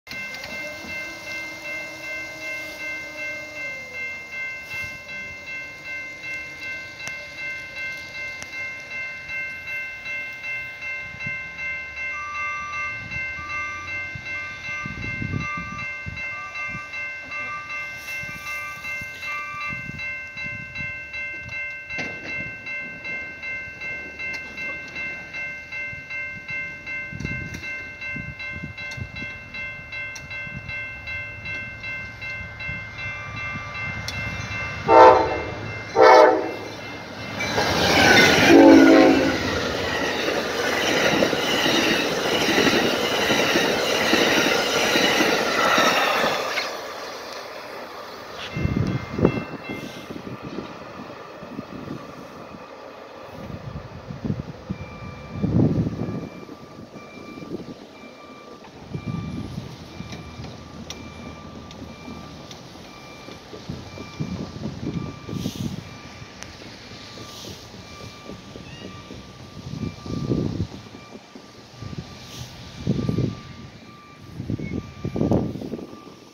Fast Amtrak In The Snow Zipping Past Longcommon Road Crossing
Amtrak 45 & 76, both GE P42DC's, kicking up a blizzard on it's way out of Chicago past Longcommon Road crossing, Riverside IL heading to Sunny California!